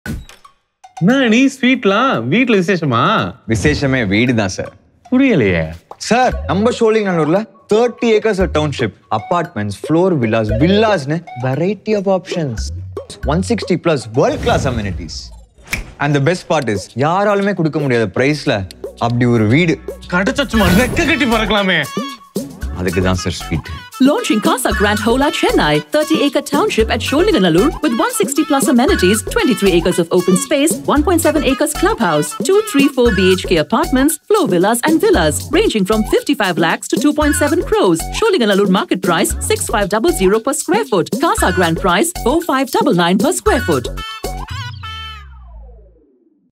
Casagrand Hola Chennai | TVC
Experience The Next Level Of Ultimate Living !!!

Casagrand Holachennai in Sholinganallur is a mesmerizing enclave spread across 30 acres, featuring 1800+ homes, each uniquely designed. From modern apartments to charming villas, every residence is a masterpiece with contemporary touches like Italian marble and large windows. The community is adorned with grand pools and an array of amenities, ensuring a lifestyle of comfort and leisure.

Regular community events and gatherings weave a tight-knit bond among residents, creating a symphony of shared joy and relaxation, Set amidst the vibrant pulse of Sholinganallur, Casagrand Holachennai exudes a welcoming ambiance with its lively colors, offering residents a sanctuary where they can embrace life’s pleasures fully and find true contentment.

RERA number – TN/29/Building/0352/2024